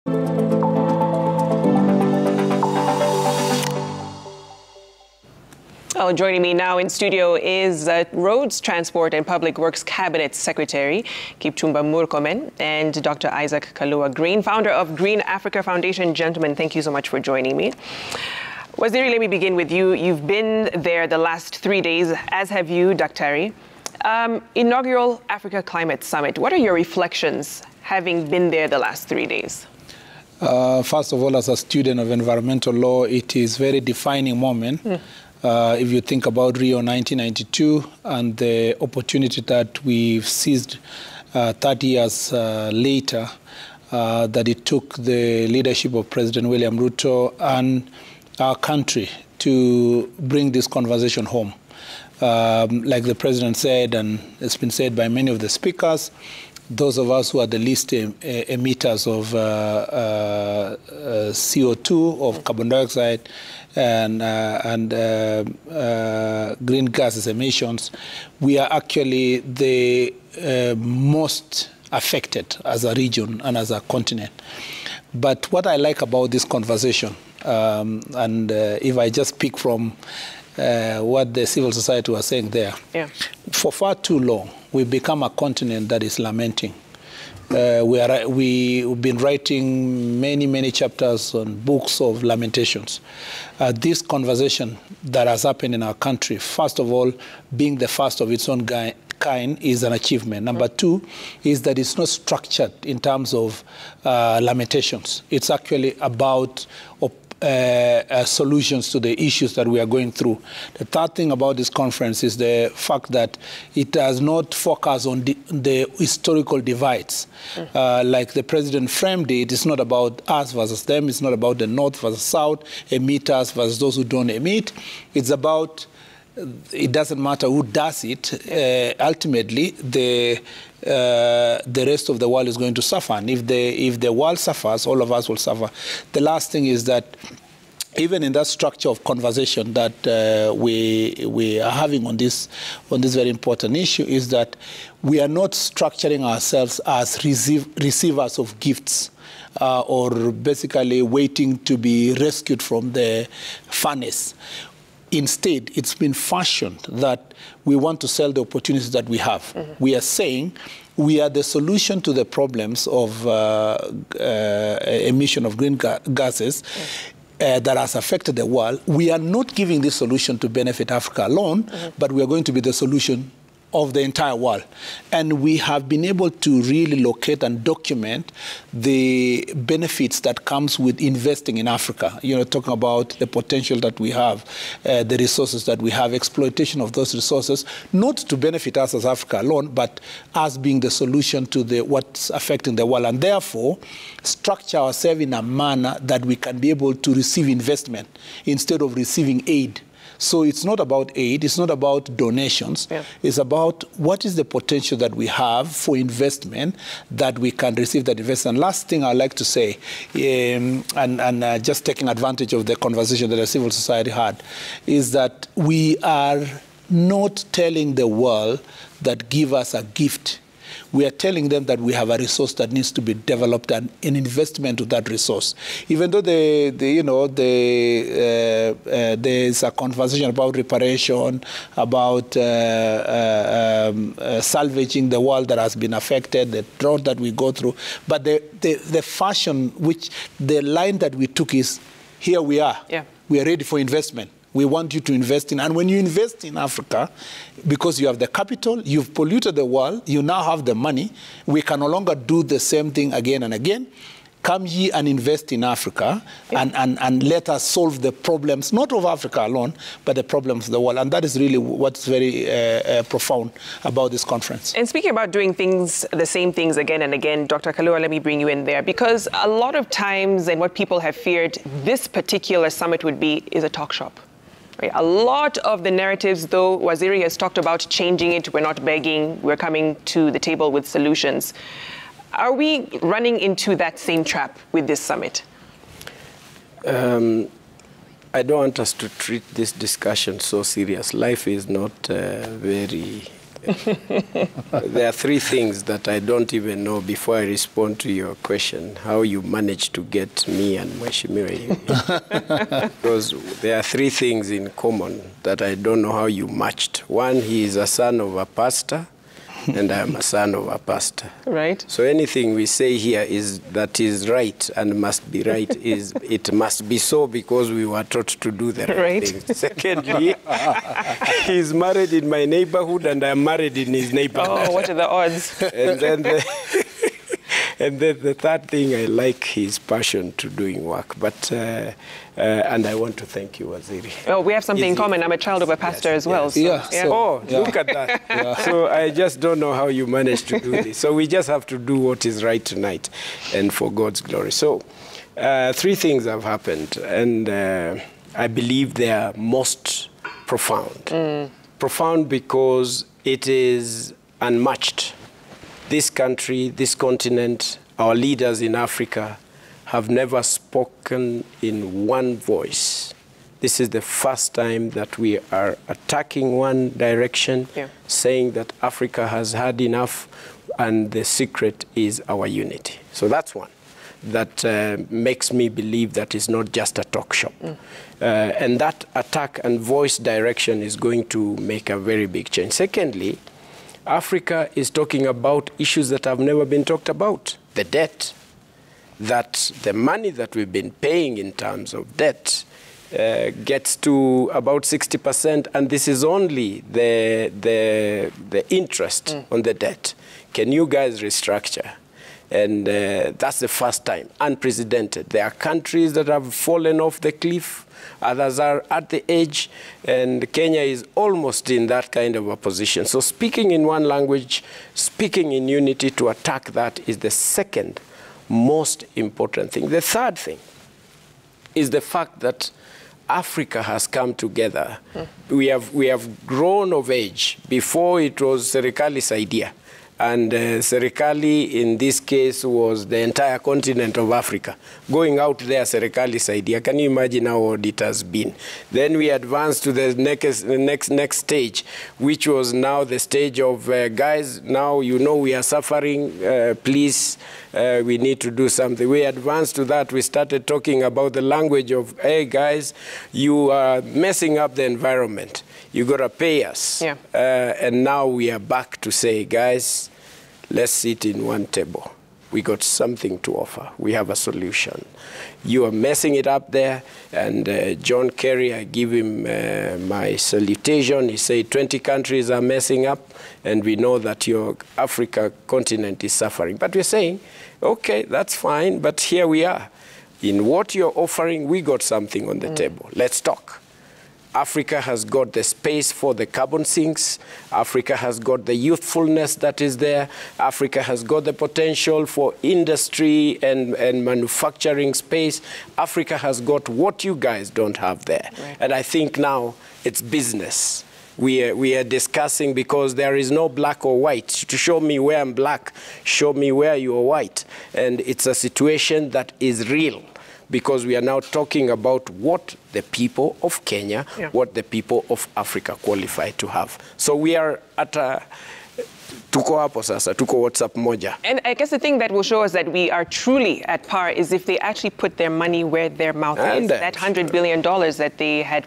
| JKLive | Road to Green Transport [Part 1]
41 percent Nairobians use public transport
40 percent of Nairobians walk
13 percent of Nairobians use private means
Gov't unveil plans for nationwide e-bike scheme
Gov't to accelerate transition to e-mobility
E-mobility race heats up in East Africa